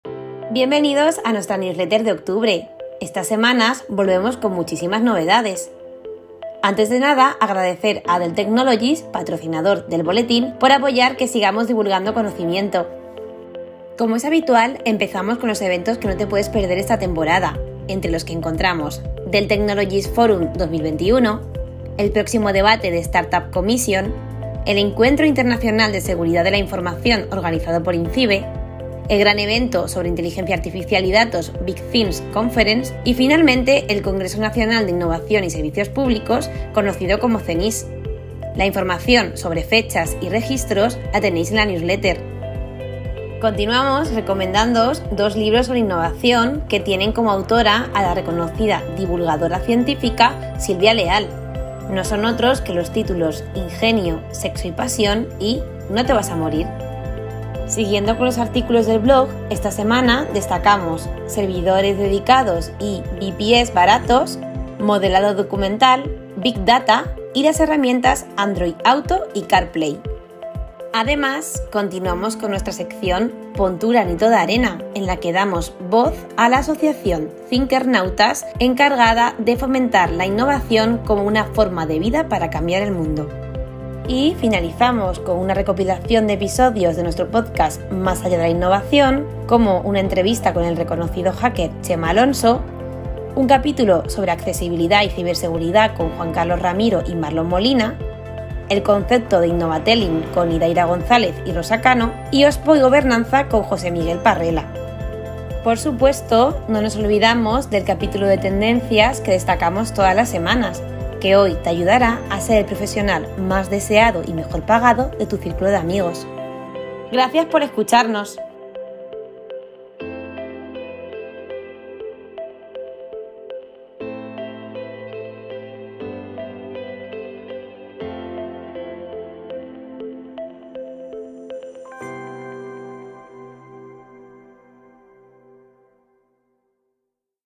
Newsletter OpenExpo Europe - T21E13- Una newsletter de suerte: Startups, innovación, ciberseguridad
🔴 Selección de contenido IT - 13ª EDICIÓN Newsletter OpenExpo

- Próximos eventos y webinars de julio: Ciberseguridad, Big Data, Customer Experience, Inteligencia Artificial, Innovación pública, cloud...

- Startup Commission: Conversaciones entre administración pública y startups

- Tu biblioteca: Innovación y transformación digital con Silvia Leal

- Blog: Servidores dedicados, VPS, modelado documental, datos, Car Play y Android Auto

- Pon tu granito de arena: Thinkernautas, por la innovación de los negocios

- Podcast - Más Allá de la Innovación: Entrevista con Chema Alonso, ciberseguridad y accesibilidad, Innova-telling, OSPO...

- Tendencias con Silvia Leal: Cómo ser el profesional más deseado y mejor pagado de tu grupo de amigos

- Podcast recomendado: El Cielo Está Enladrillado

- Talento digital y OpenJobs: Data protection, Cloud, Kubernetes, IA.

- Vídeos destacados: Mesa redonda - Ventajas y posibilidades del uso de Kubernetes

*Newsletter del 19 de octubre de 2021